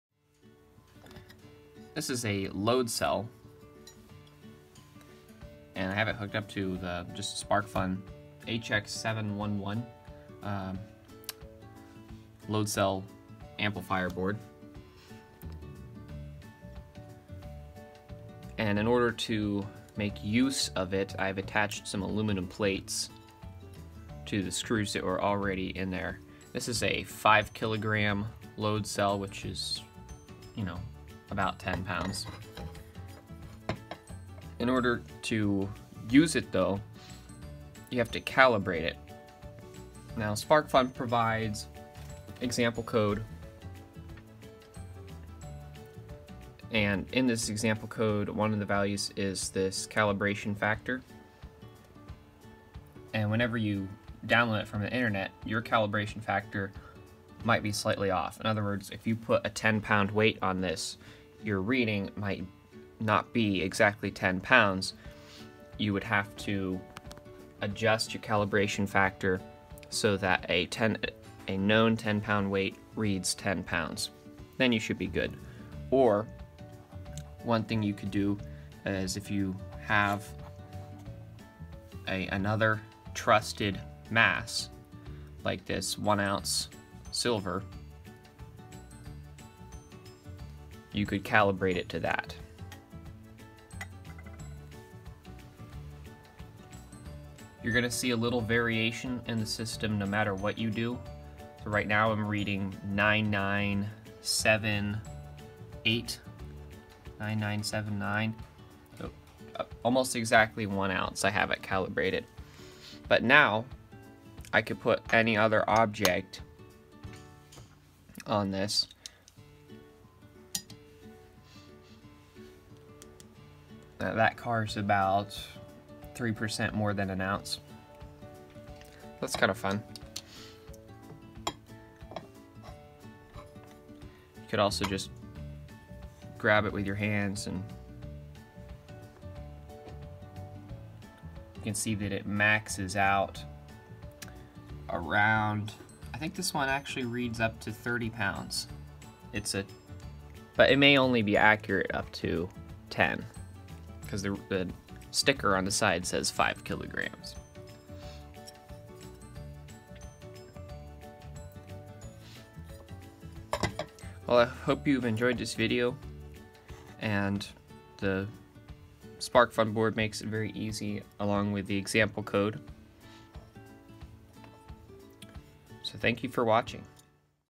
build ACCURATE scale with an arduino and a hx711 load cell
In this video we build a scale using a load cell. We use the Arduino platform to at the micro processor for the brains of the unit. We use a troy ounce of silver to calibrate the scale.
HX711 spark fun load cell.